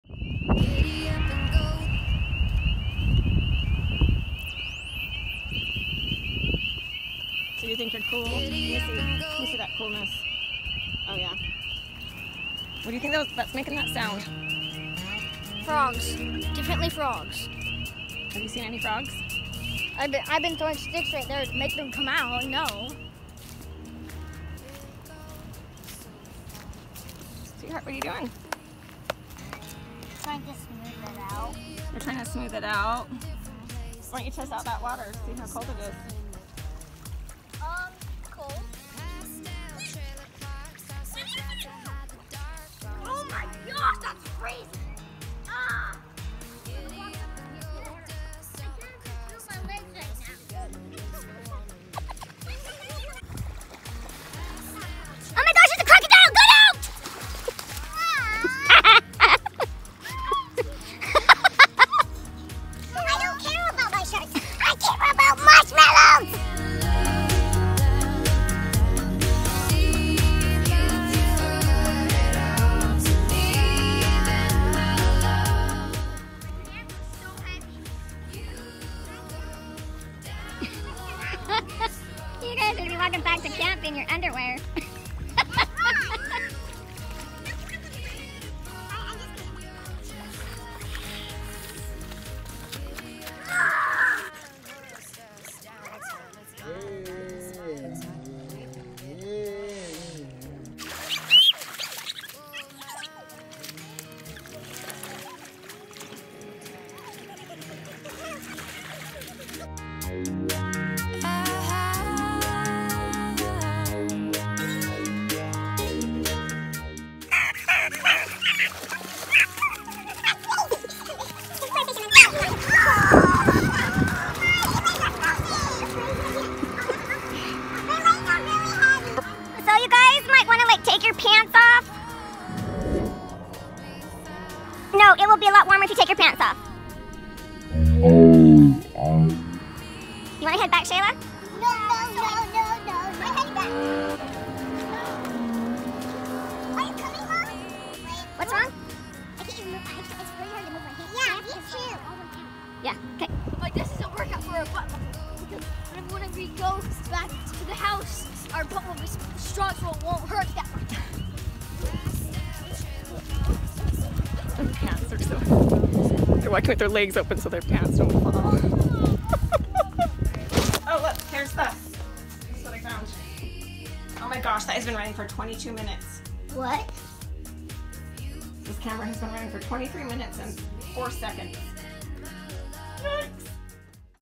2021_SpringBreak_Arkansas-Frogs
Eureka Springs…. Loved this place!  We beat the crowds and had the place to ourselves… roamed and explored all day! Everyday!